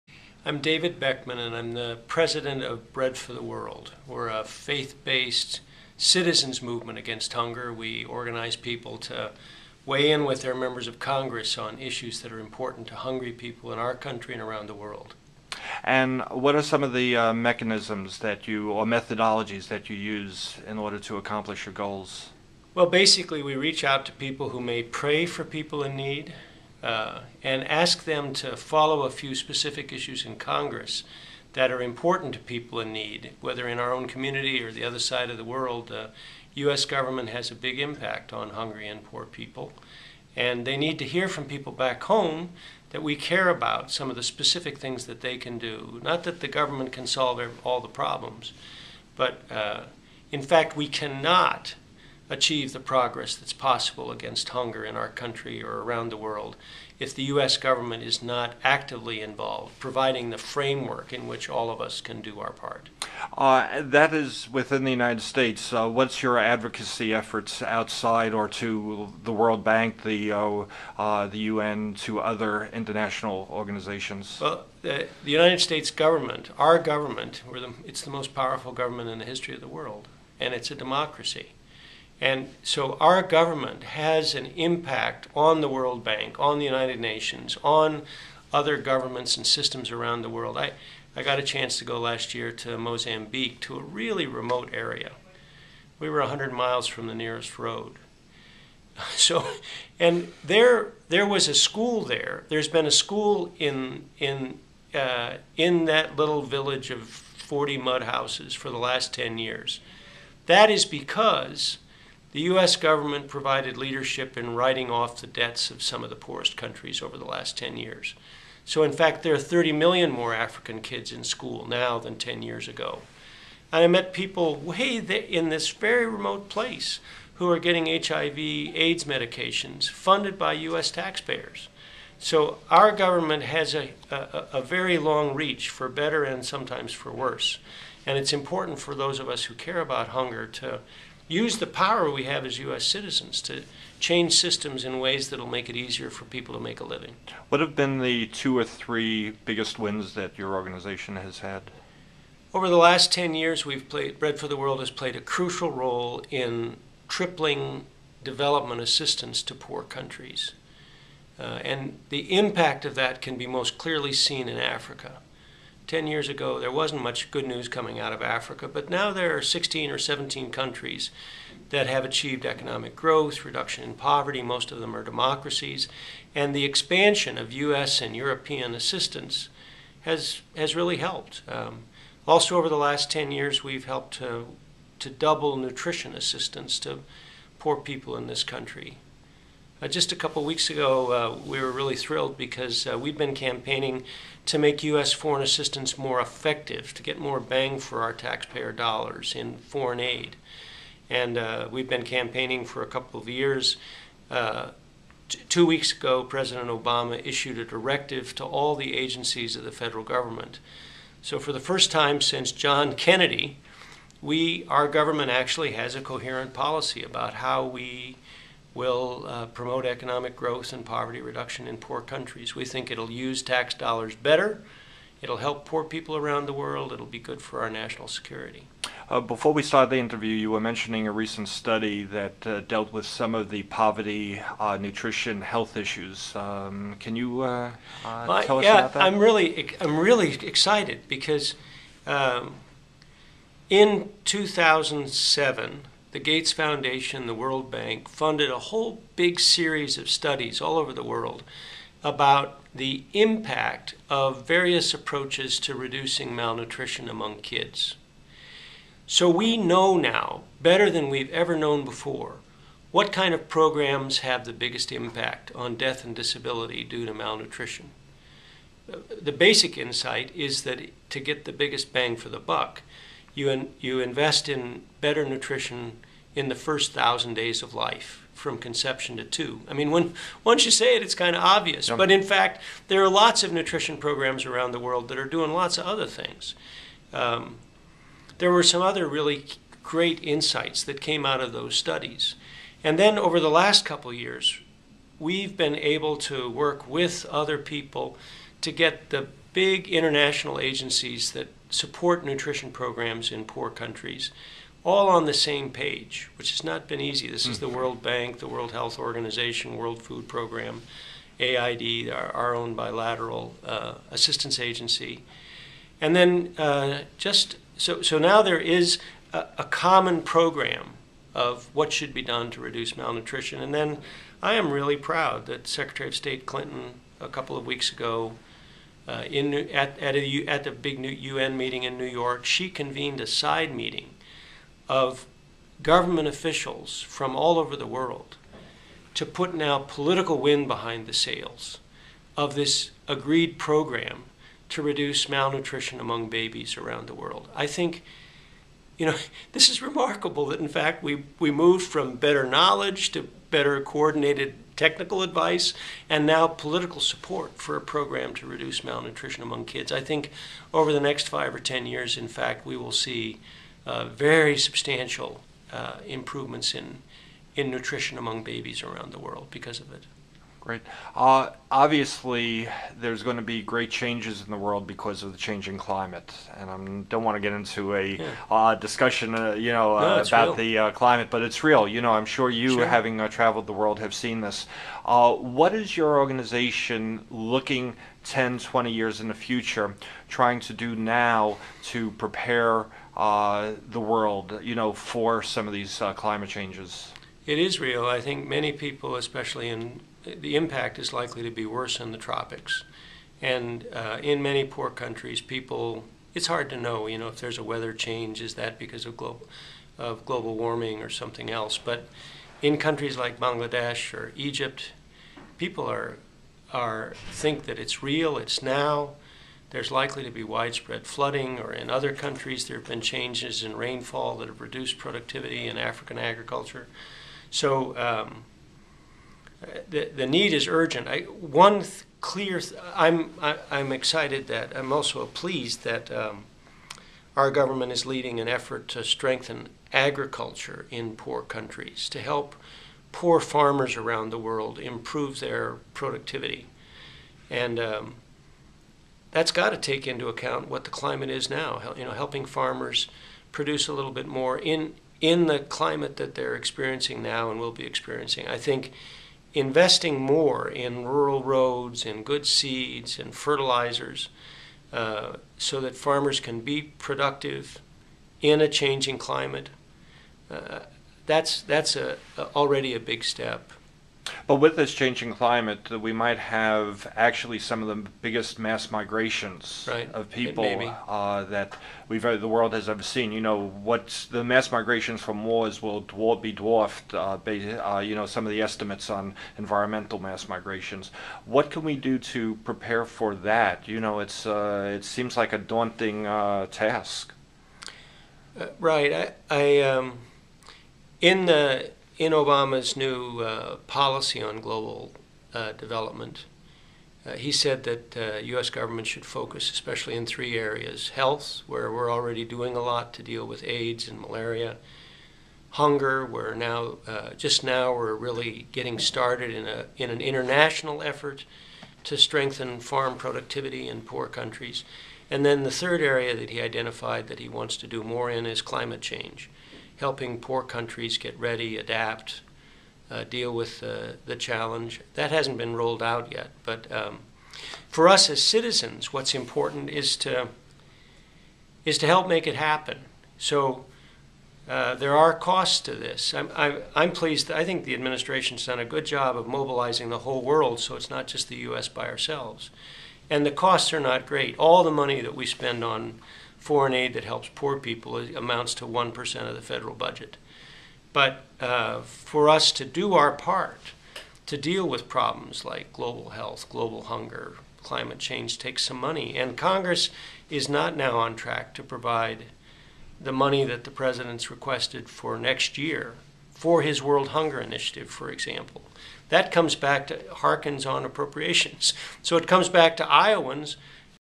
David Beckmann Inteview at World Food Prize part 1
Rev. David Beckmann one of two Laureates for the 2010 World Food Prize give an interview on Bread for the World and Hunger in the world.